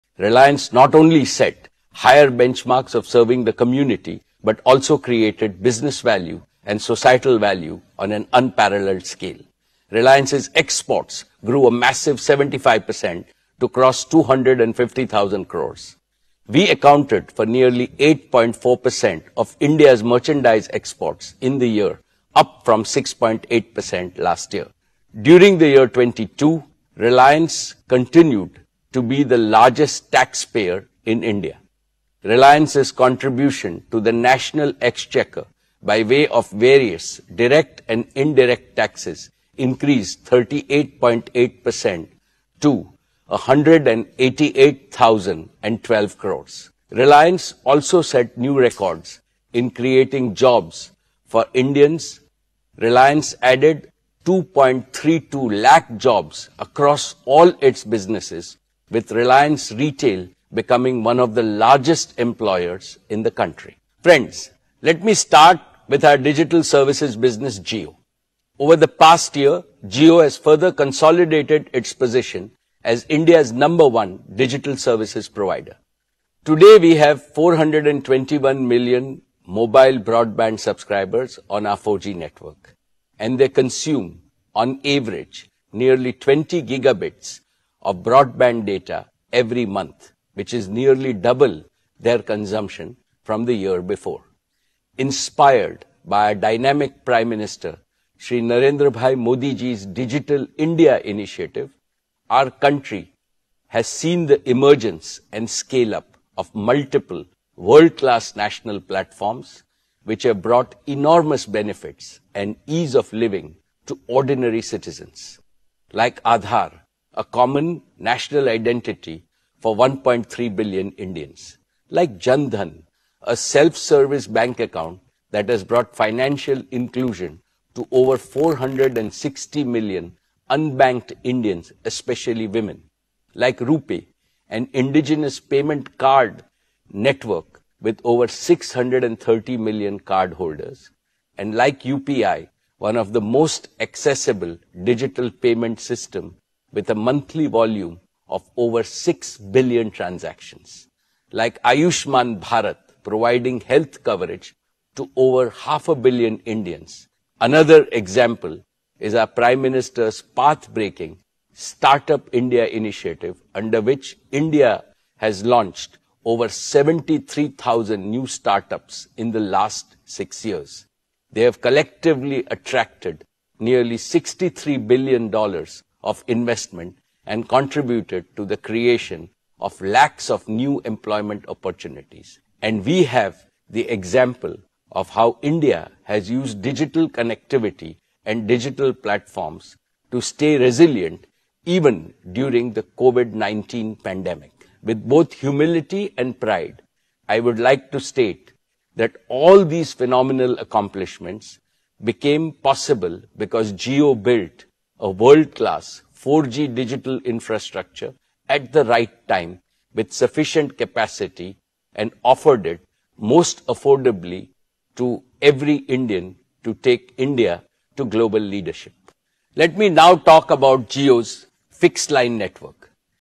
Reliance AGM 2022: Mukesh Ambaniની AGM દરમિયાન મહત્વની જાહેરાત | Gujarat News
We are ready to play an important role in the development of India - Mukesh Ambani: Mukesh Ambani

ભારતના વિકાસમાં મહત્ત્વની ભૂમિકા ભજવવા માટે અમે તૈયાર છીએ- મુકેશ અંબાણી: Mukesh Ambani